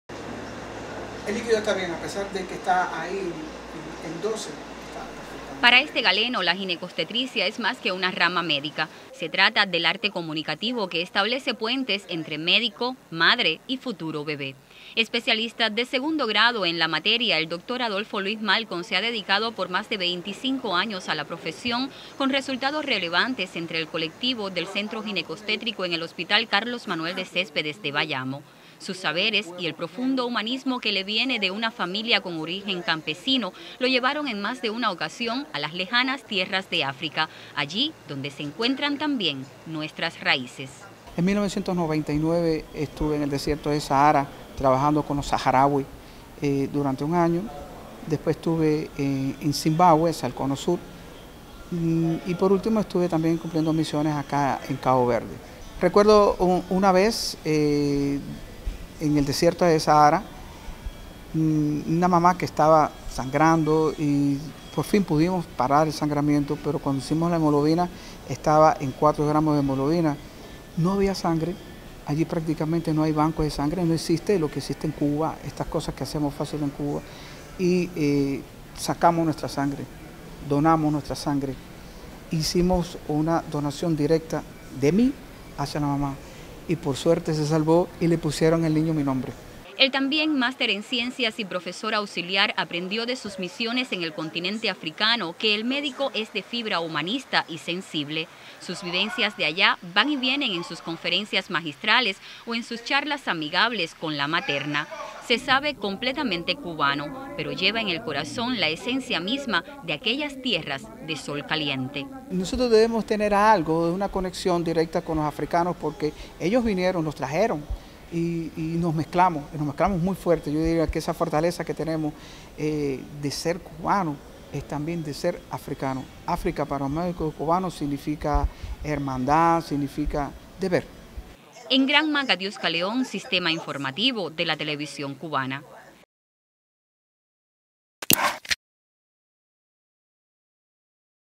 Dr. Malcon: África para los médicos cubanos significa hermandad
El 25 de mayo de cada año se celebra el Día de África recordando la fundación de la Organización para la Unidad Africana organizada con el fin de promover la unidad y solidaridad en esa región del planeta.
Por muchos años Cuba se ha involucrado con esta celebración, no solo recordando la fecha establecida, sino apostando desde el internacionalismo por un continente Africano con mejores condiciones para sus habitantes. La salud pública ha sido la misión cubana de mayor impacto en esas tierras, así lo recuerda el doctor bayamés Adolfo Luis Malcon, quien en tres oportunidades viajó a África para dar su aporte solidario en bien de la salud de su gente.

Le puede interesar: Los cubanos celebramos el día de África con un aporte solidario